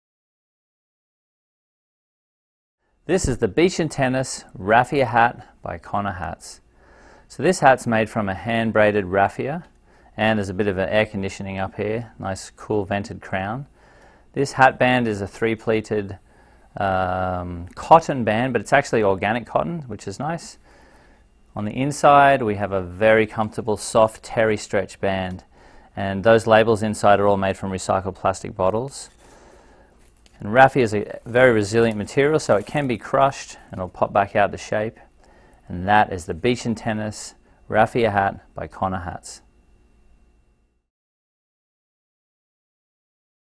Beach And Tennis Raffia Hat SKU# F5132
This lightweight safari style hat features a cool mesh crown and a handsome organic cotton pleated hat band.